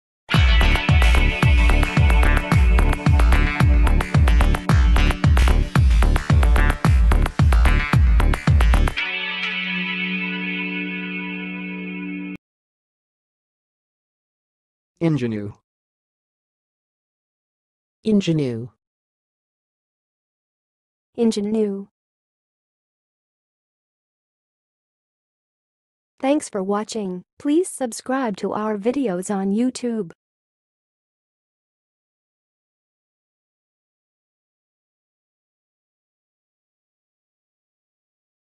How to say "ingénue"! (High Quality Voices)
Watch in this video how to say and pronounce "ingénue"!